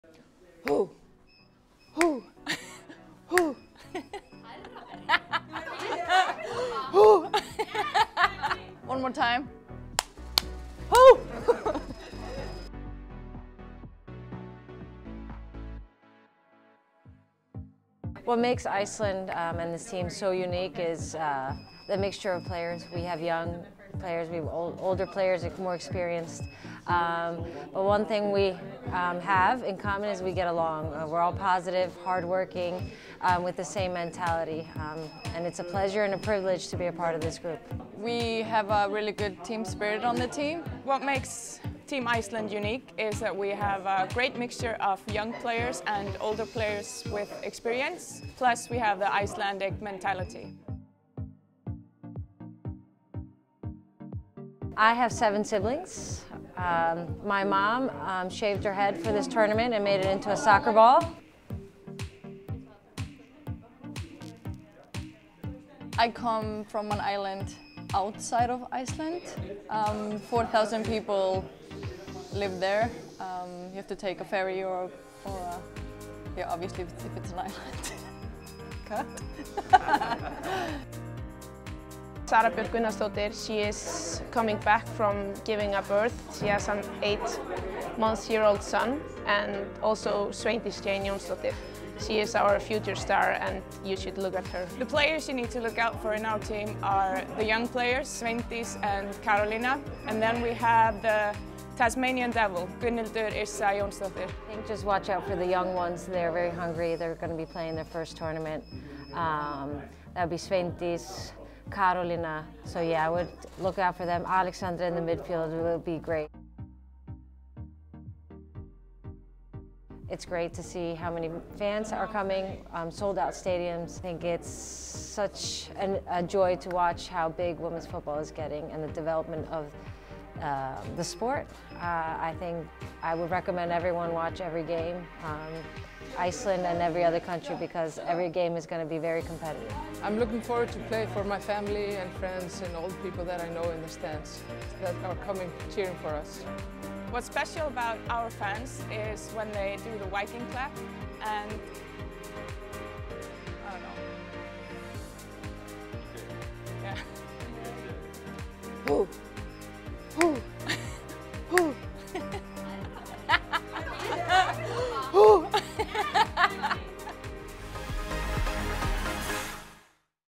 ICELAND Inside View: JÓNSDÓTTIR, THORVALDSDÓTTIR, GÍSLADÓTTIR, VIDARSDOTTIR | WEURO 2022
Iceland stars Gunnhildur Jónsdóttir, Berglind Thorvaldsdóttir, Hallbera Gísladóttir and Elisa Vidarsdottir analyse their side's strengths, stars and hopes ahead of UEFA Women's EURO 2022.